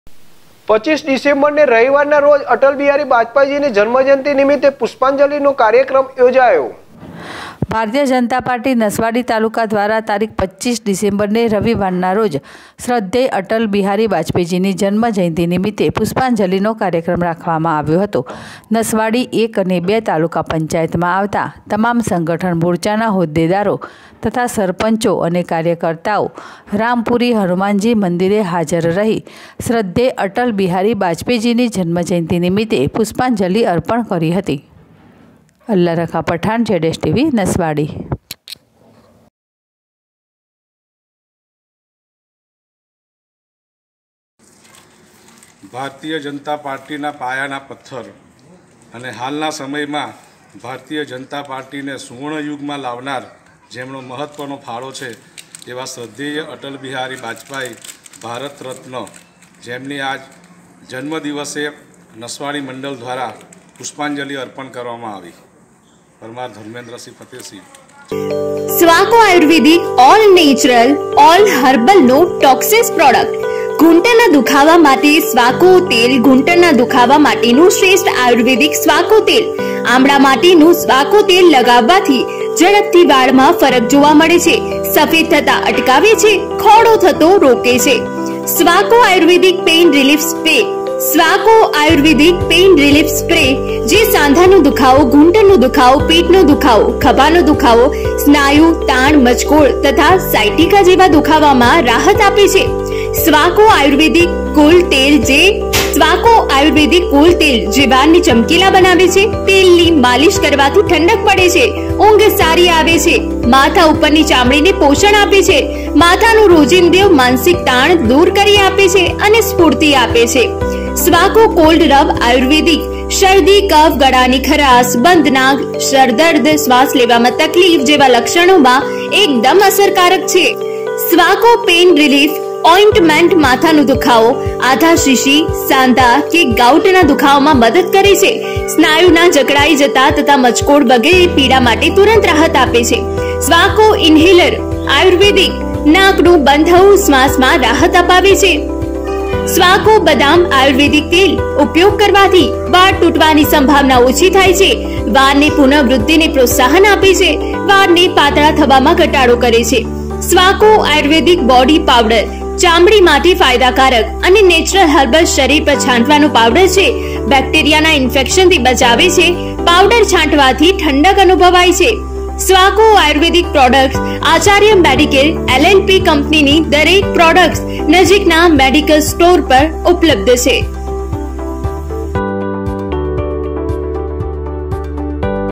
છોટાઉદેપુર ૨૫ ડિસેમ્બર ને રવિવાર ના રોજ શ્રદ્ધેય અટલ બિહારી બાજપાઈજી ની જન્મજયંતિ
છોટાઉદેપુર ૨૫ ડિસેમ્બર ને રવિવાર ના રોજ શ્રદ્ધેય અટલ બિહારી બાજપાઈજી ની જન્મજયંતિ નિમિત્તે પુષ્પાંજલિ નો કાર્યક્રમ રાખવામાં આવ્યો